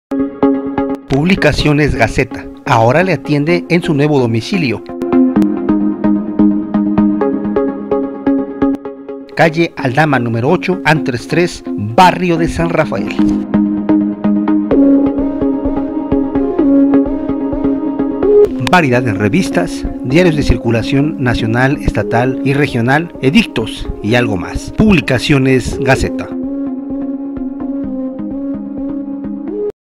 Publicaciones Gaceta de Teziutlán ahora tiene nuevo domicilio
Calle Aldama número 8, Barrio de San Rafael.